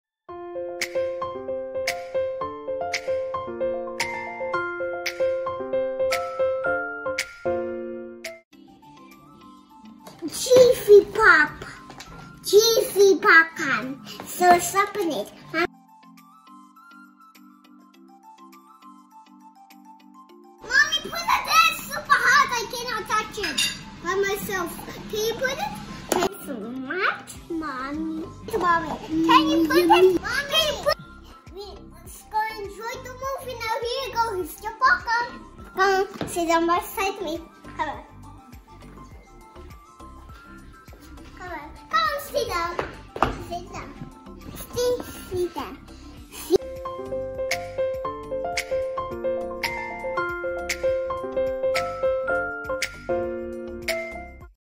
Jiffy pop
Remember the jiffy popcorn that you had to shake back and forth over the burner so it could pop the popcorn? ( if you don't  know ,then Probably it was before your time 😂( The old school style before the microwave popcorn! ) 😂
Got some sweet memory of this when we were kids! 🥰